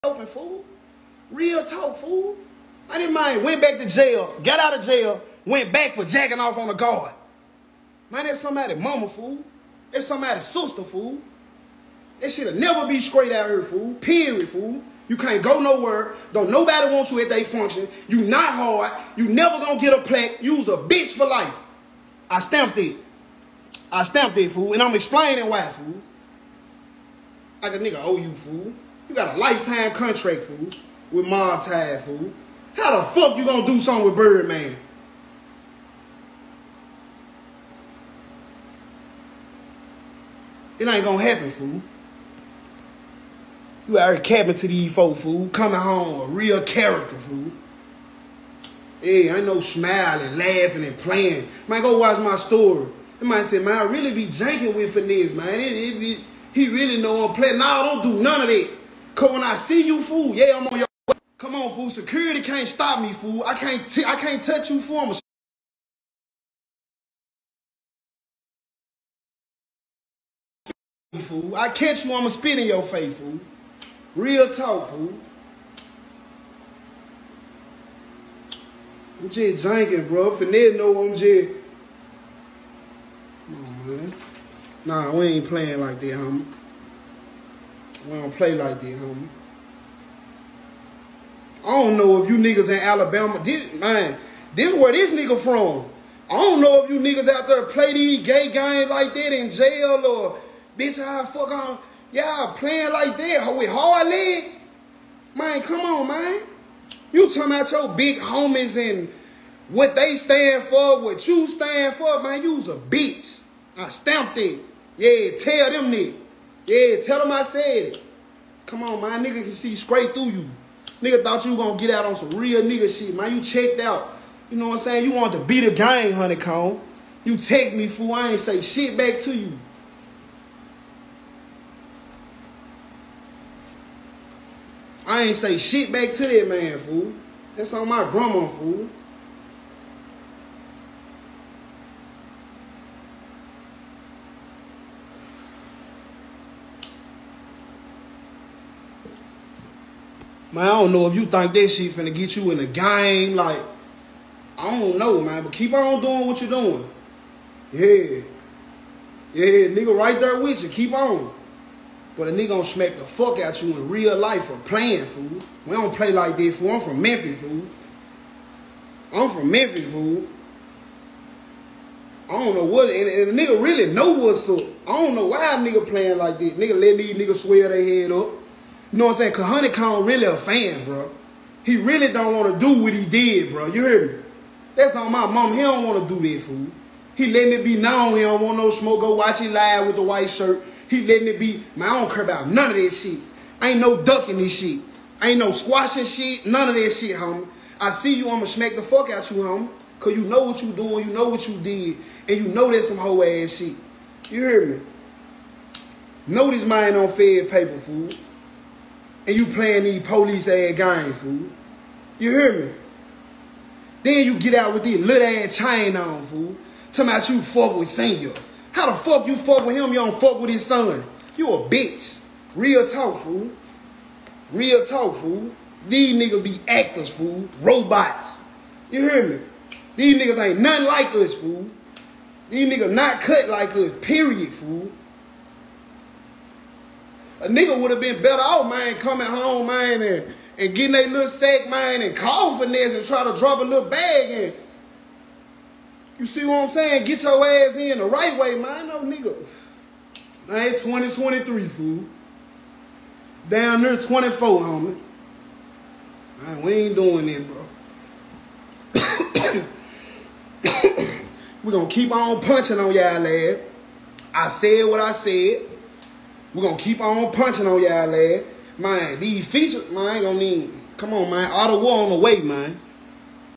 Finesse2tymes brother says he’s smacking Honeykomb Brazy for dissing 👀 “ain’t no squashing beef”😳
finesse2tymes brother says there’s no squashing beef with honeykombbrazy and he’s smacking Brazy when he see him..Also speaks on birdman